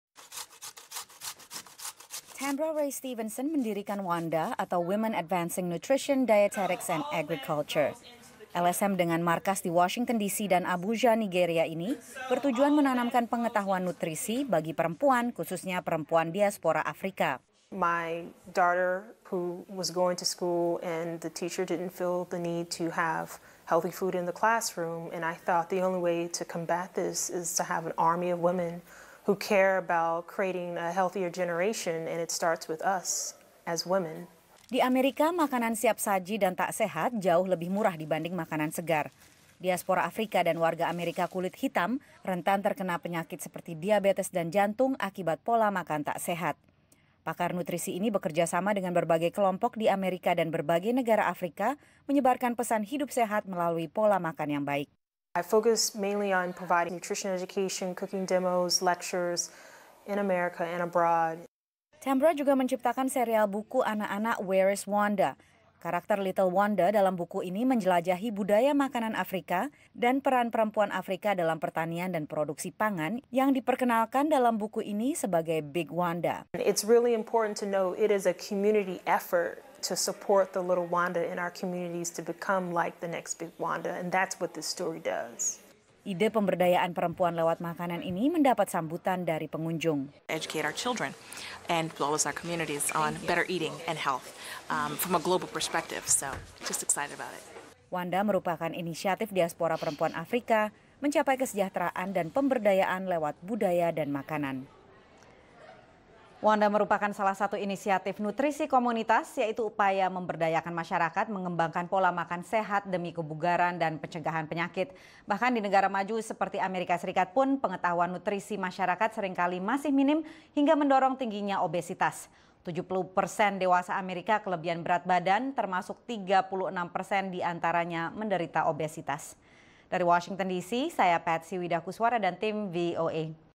Program Tingkatkan Pengetahuan Nutrisi Komunitas - Laporan VOA
Kurangnya pemahaman nutrisi merugikan masyakarat karena pola makan tak sehat menimbulkan penyakit dan mengurangi produktivitas. Upaya pemerintah dinilai tak cukup, khususnya bagi kalangan diaspora Afrika di Amerika. Hal ini memicu prakarsa komunitas yang dimotori seorang perempuan Diaspora Nigeria.